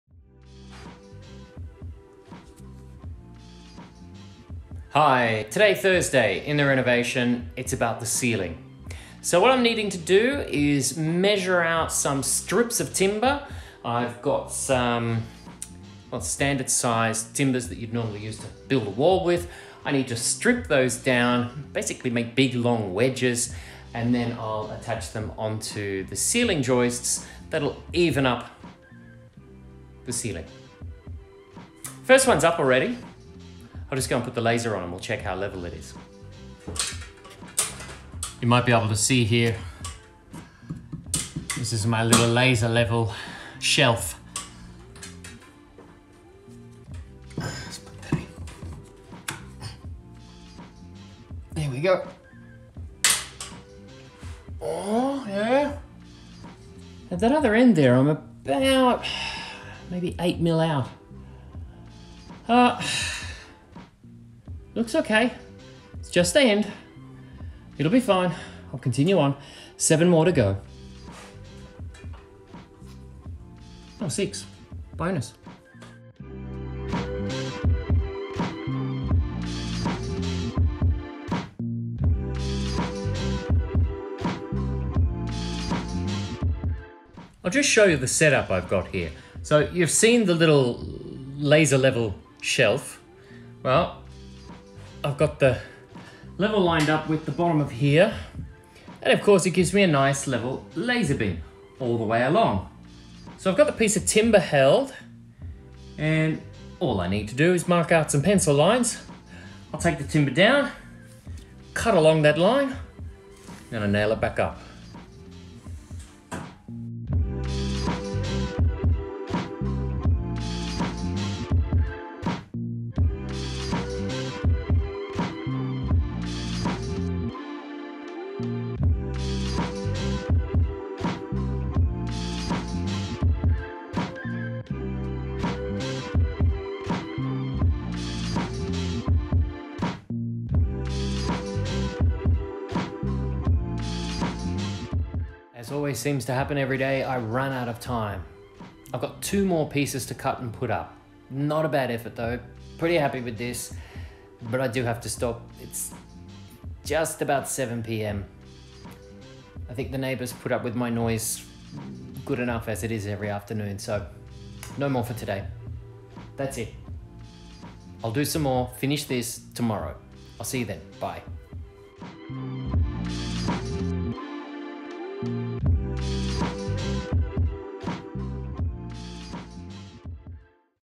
Renovation: leveling the ceiling.
Using a suspended laser level to mark and cut timber strips.